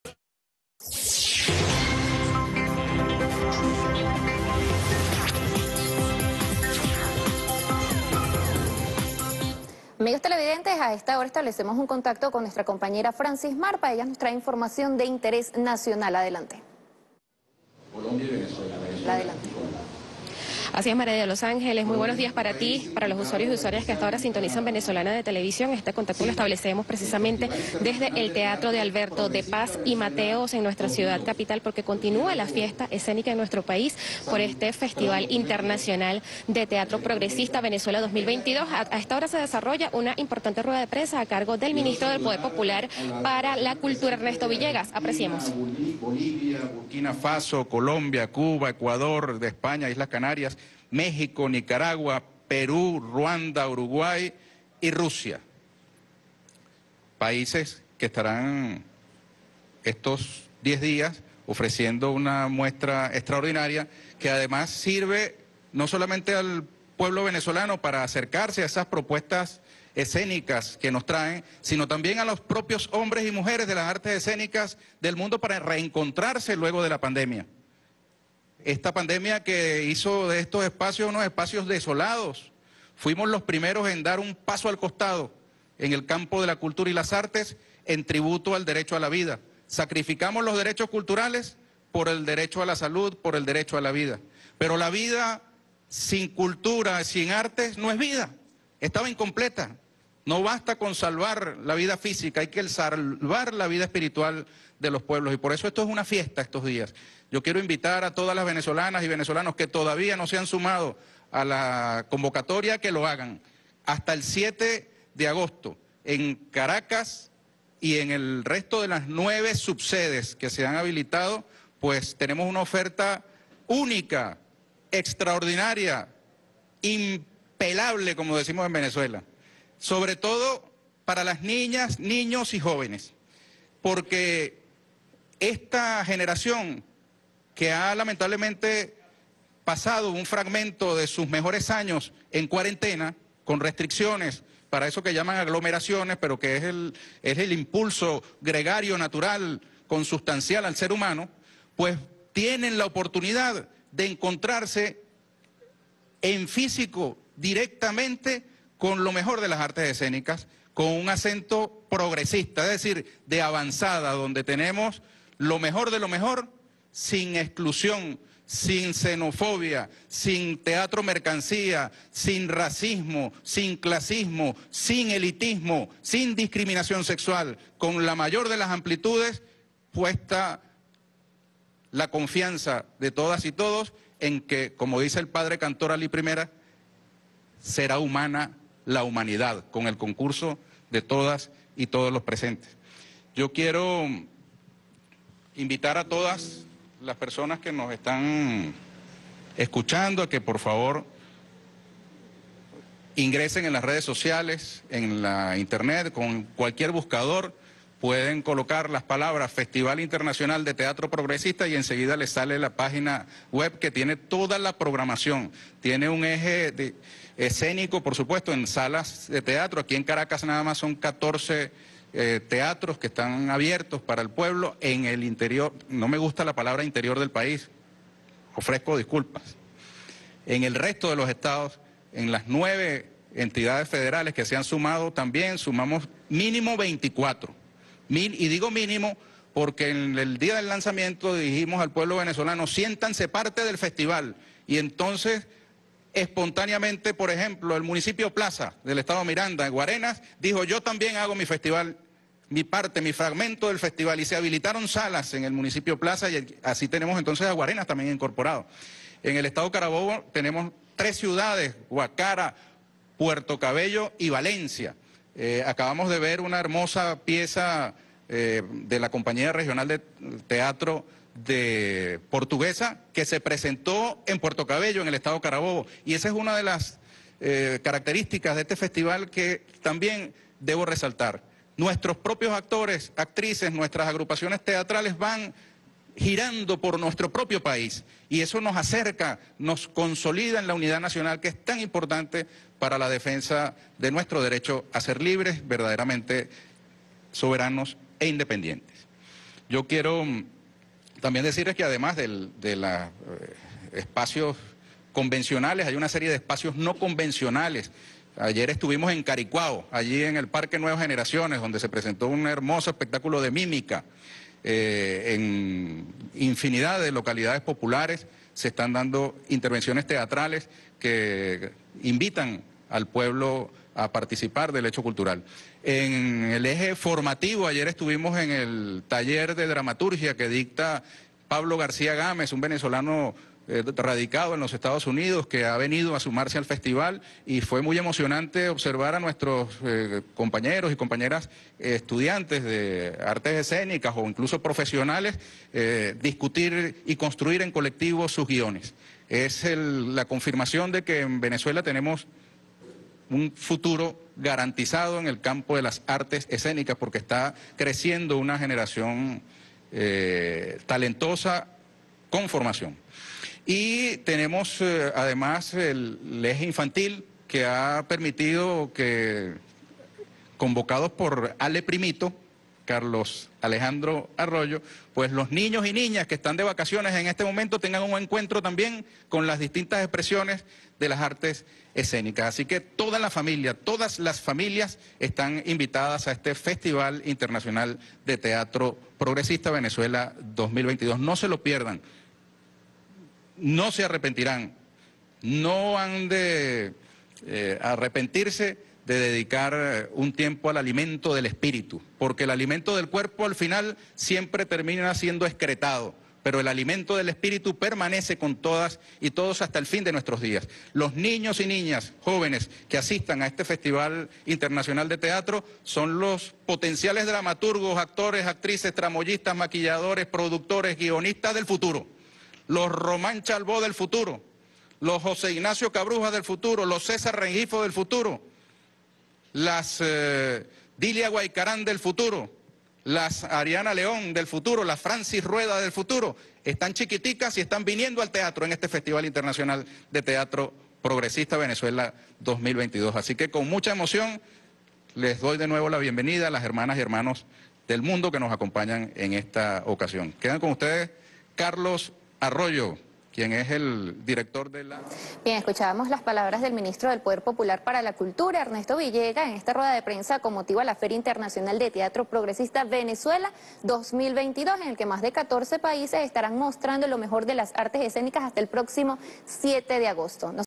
Ernesto Villegas en rueda de prensa por el Festival de Teatro Progresista, 2 de agosto de 2022
Desde el Teatro Alberto de Paz y Mateos en Caracas, el ministro de Cultura de Venezuela, Ernesto Villegas, ofrece una rueda de prensa por el Festival Internacional de Teatro Progresista este martes 2 de agosto de 2022.

USDT o Bitcoin (BEP20): 0xef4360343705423a24119205eeb1de2a10fd23f4

USDT (TRC20): TQJB86JZjF5Kt1C7f322aax4iyD6x65UcV

Petros: PFfDUW3cj1WD7cUPYg9MK2EtgG5nUgVzuc